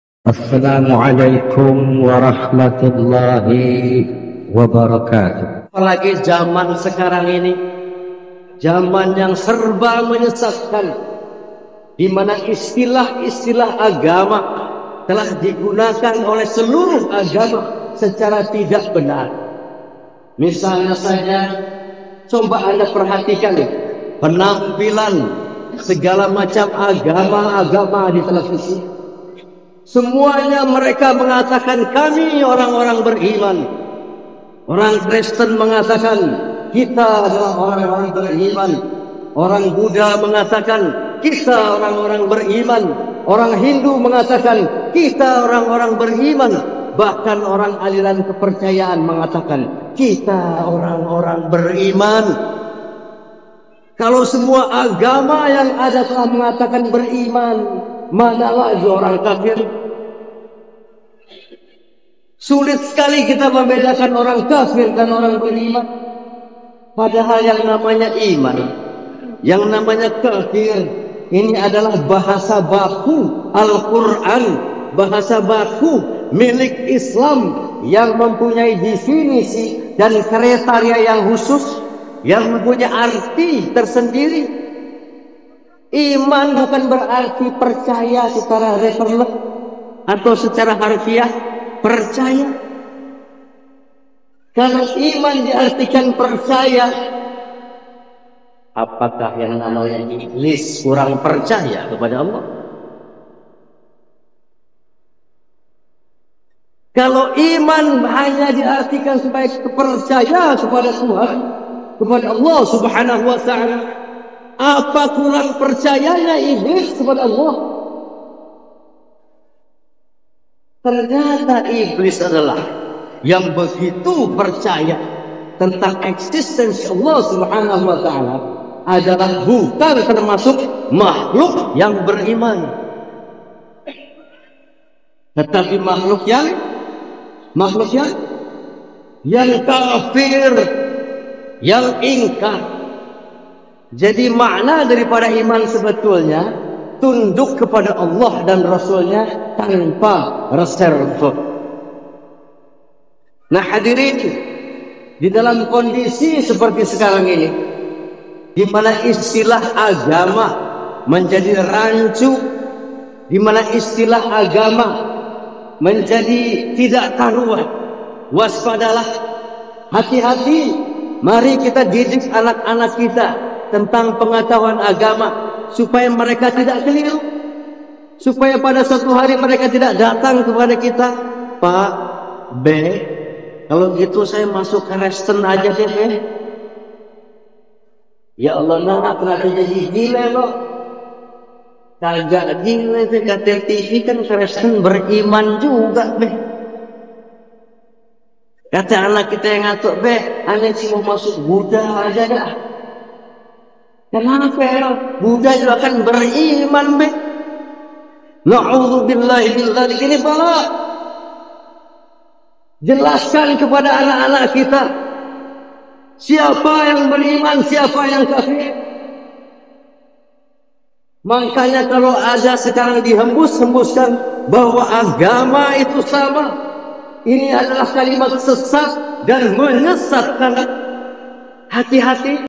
RHOMA IRAMA SUDAH BERDAKWAH DARI TAHUN 80'AN.
Rhoma Irama ceramah di Majelis Ta'lim Habib Ali Al Habsyi Kwitang, Minggu 18 Agustus 1991.